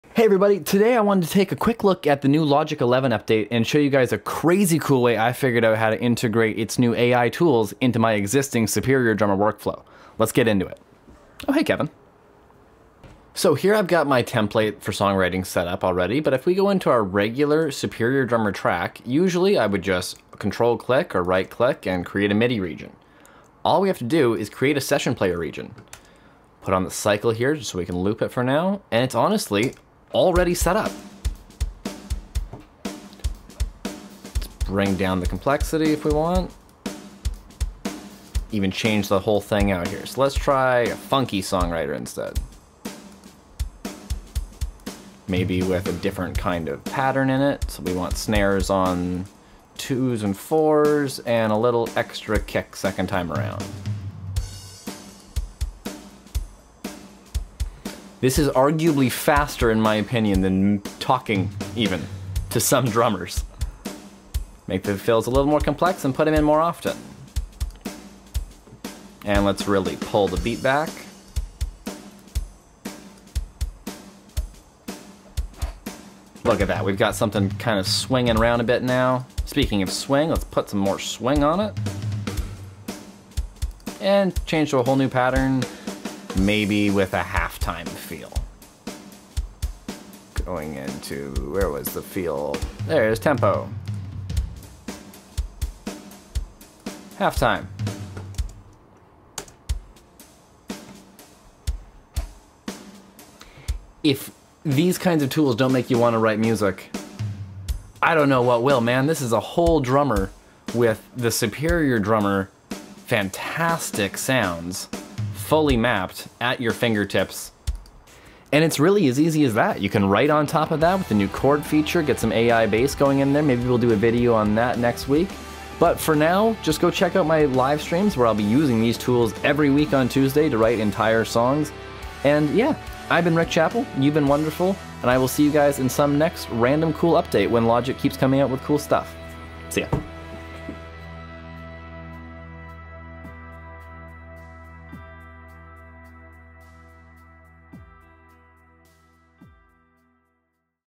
Superior Drummer 3 + Logic Pro 11 Ai Session player setup
After upgrading to the latest version of logic I've been unlocking countless ideas with these new Ai driven tools. This is just one example of how I've integrated the new session player into my existing sounds and templates.